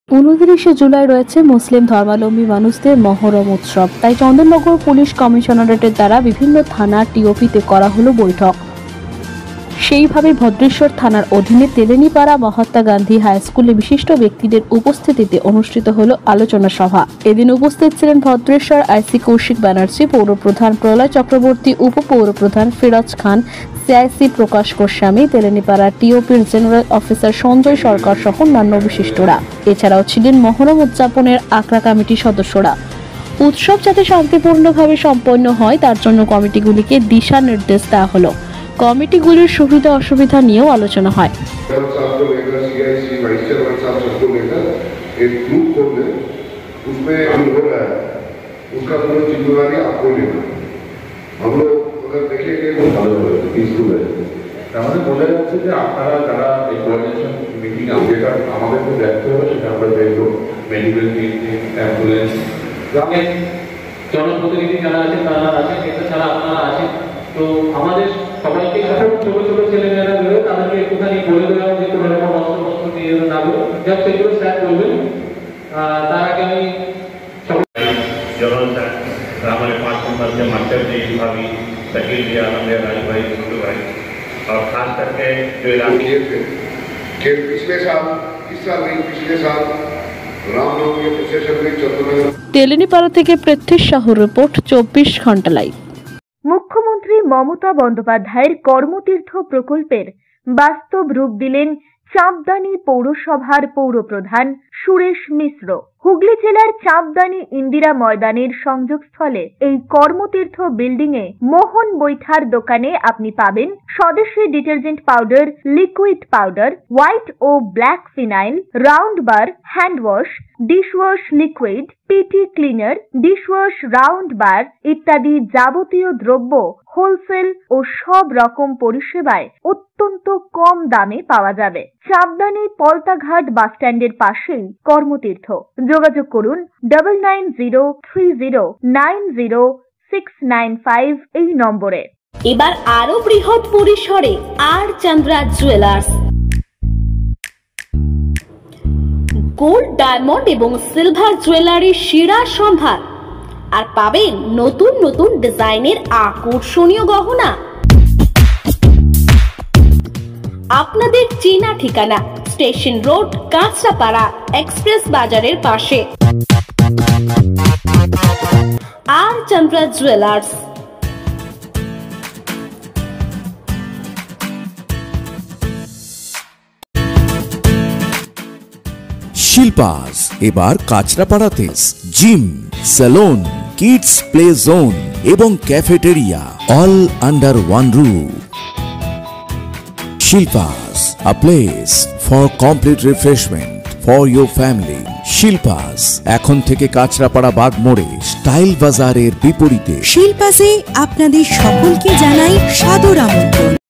তেলিনিপাড়া মহাত্মা গান্ধী হাই স্কুলে অনুষ্ঠিত হলো মহরম নিয়ে বৈঠক
ভদ্রেশ্বর থানার অধিনে তেলিনিপাড়া মহাত্মা গান্ধী হাই স্কুলে বিশিষ্ট ব্যক্তিদের উপস্থিতিতে অনুষ্ঠিত হলো আলোচনা সভা।